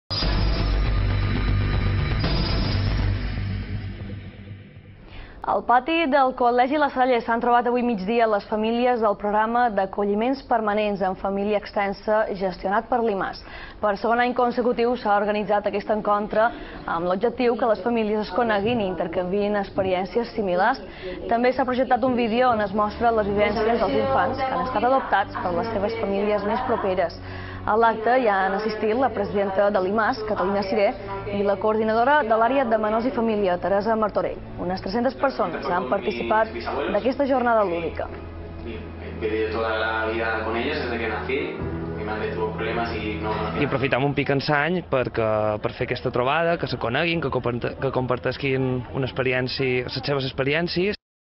From Outer Space con las familias de acogida en TV Mallorca
Pieza del informativo de TV Mallorca, donde presentamos el vídeo hecho para colaborar con ACOFAM.